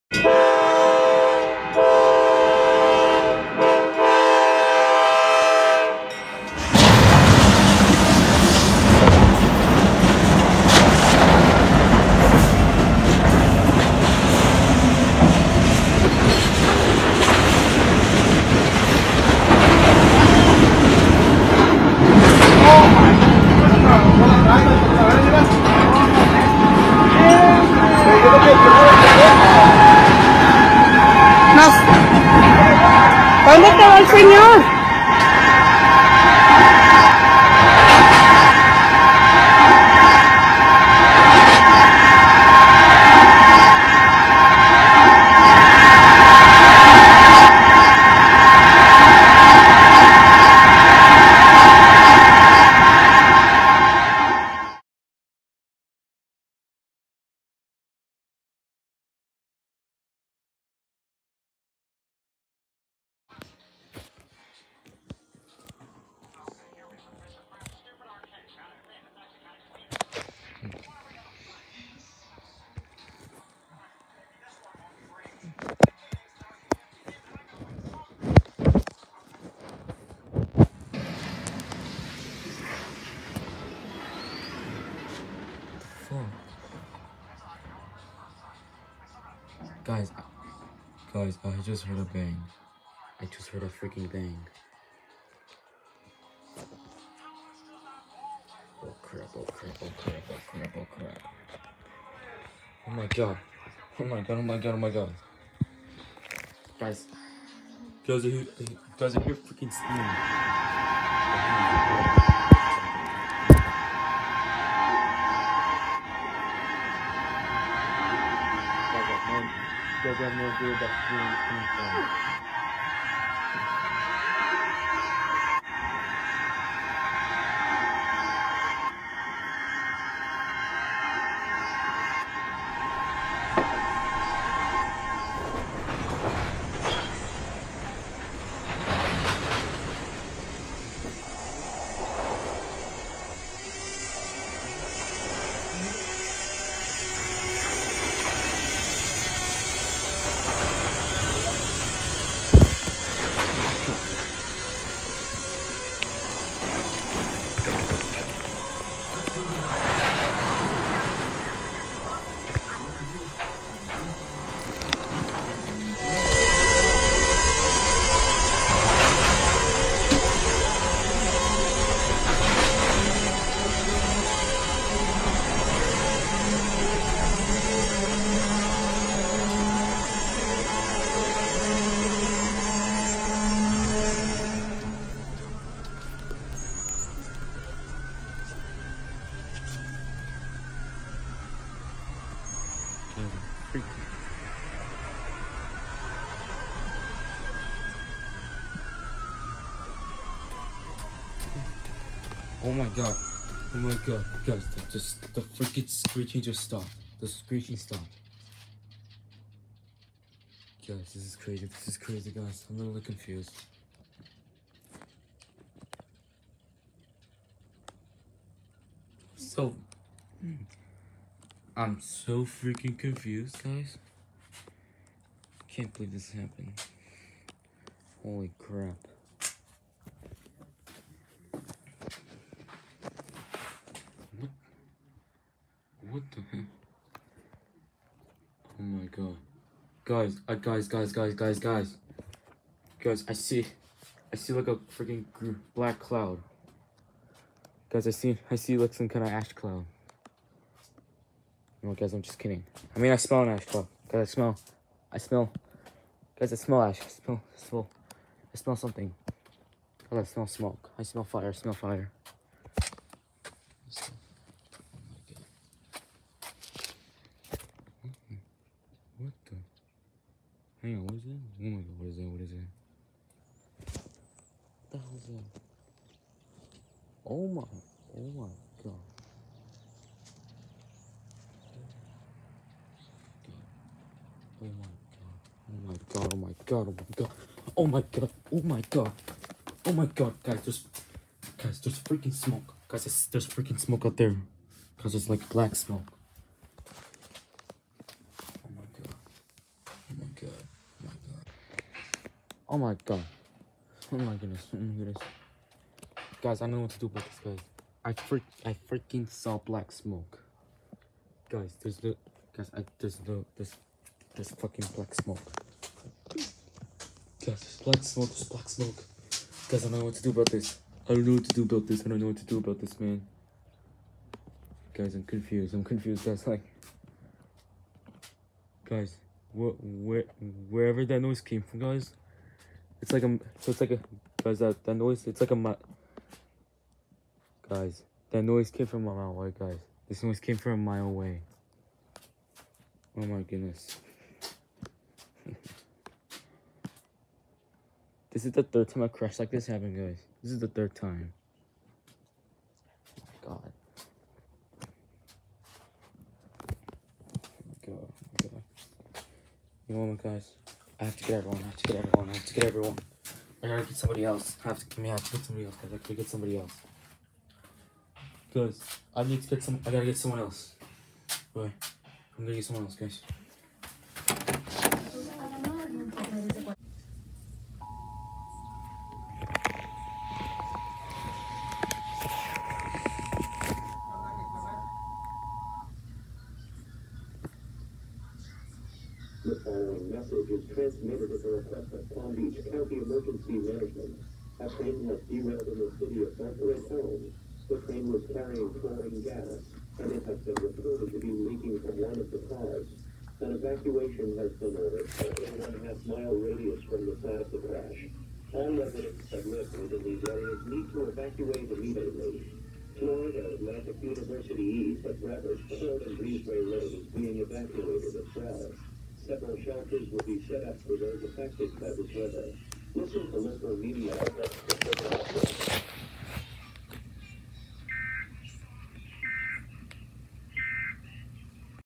KCS Manifest train derails after collision with lowboy
This happened around 10:26 AM in Ashton county, CA. I heard this bang from a mile away and I saw some black smoke.  It could’ve been something that caught on fire.. I had someone to come with to find out what made the loud bang..